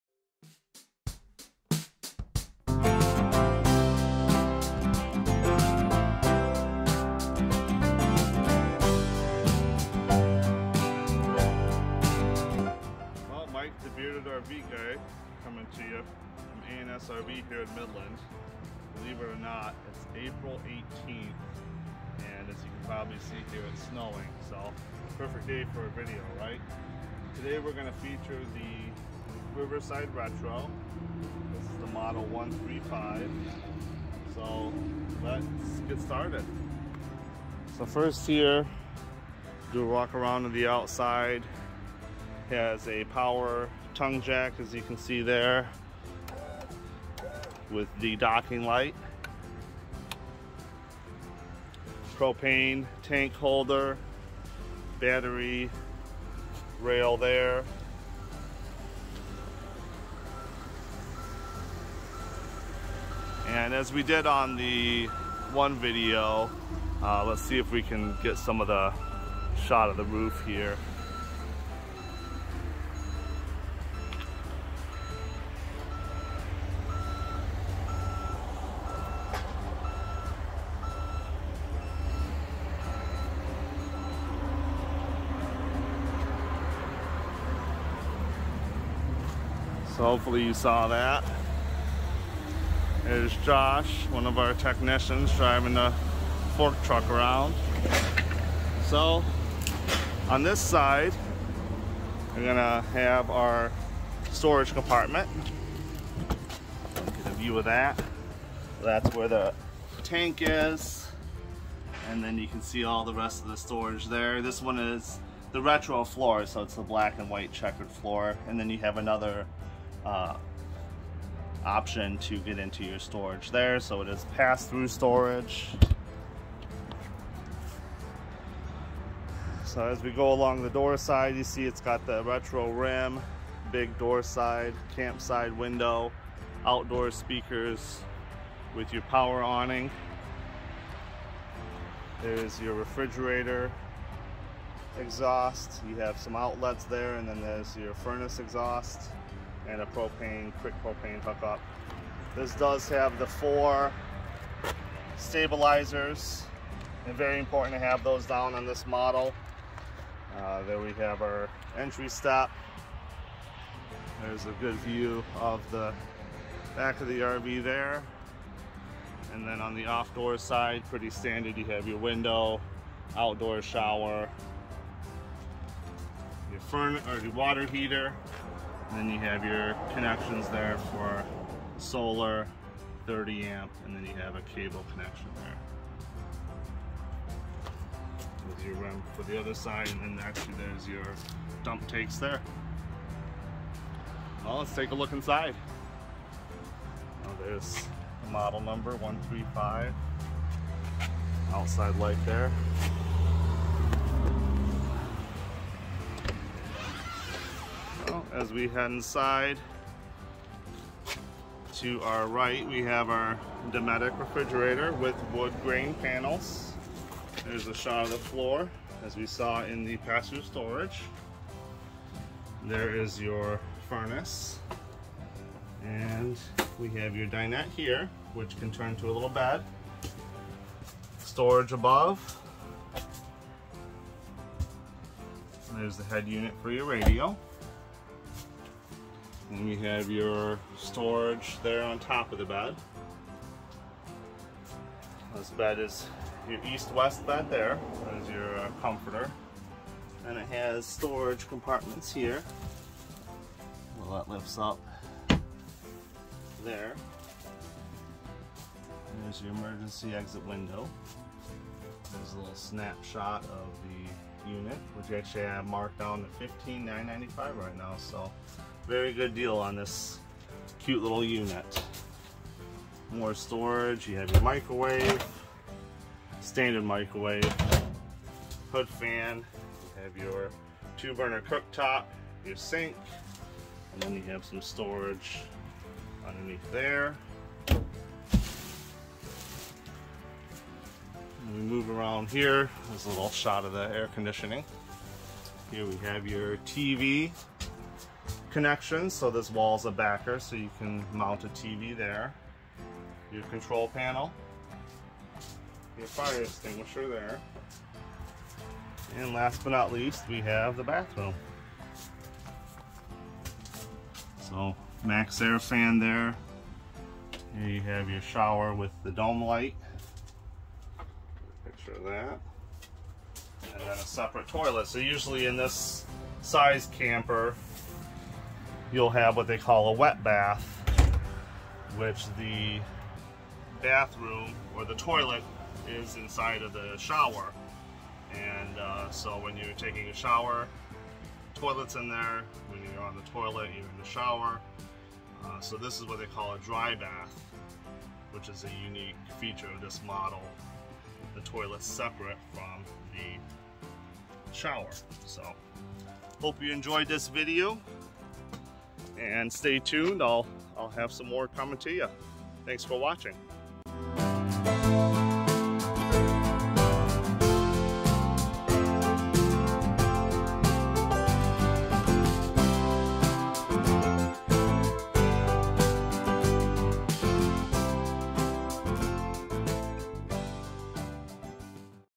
RV Tour: REAR ENTRY Retro 135 by Riverside
Now you can indulge in nostalgia without sacrificing the comfort of modern conveniences. The brand new 2022 Retro 135 by Riverside RV is light as a feather and has style in spades.

Call Mike at with any questions or to schedule a showing at A&S RV Center, 1983 Airport Rd, Midland, MI 48642.